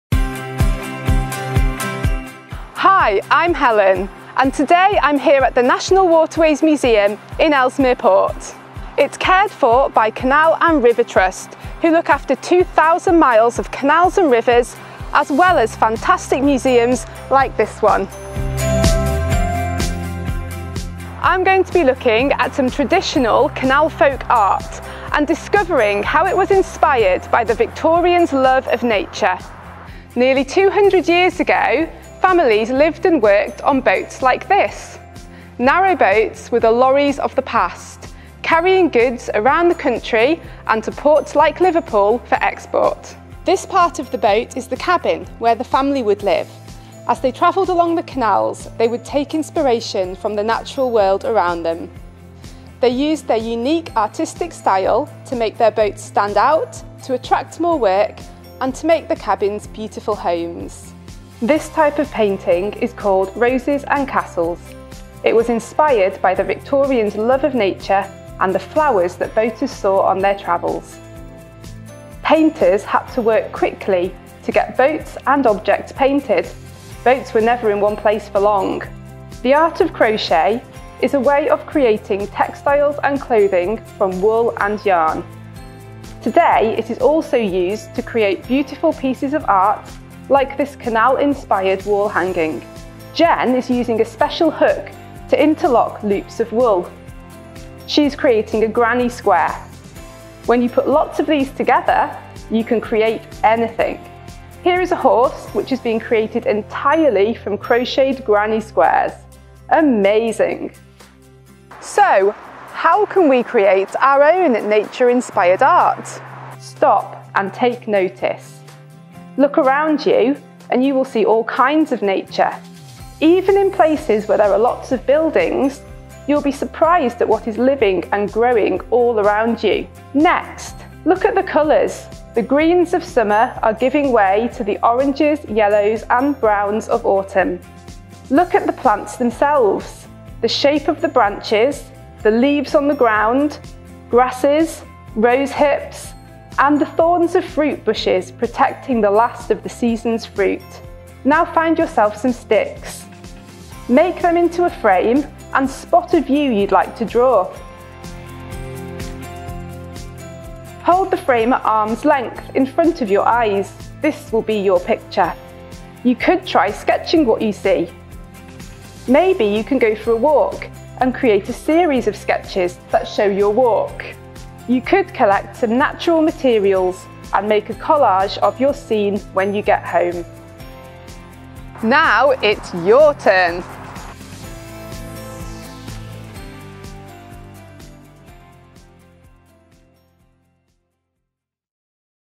Nature inspired art
Let’s learn about traditional canal art, Roses & Castles, and how it was inspired by the Victorian’s love of nature. Artists today continue to be inspired by the natural world around them, how will you be inspired by nature along our canals and rivers?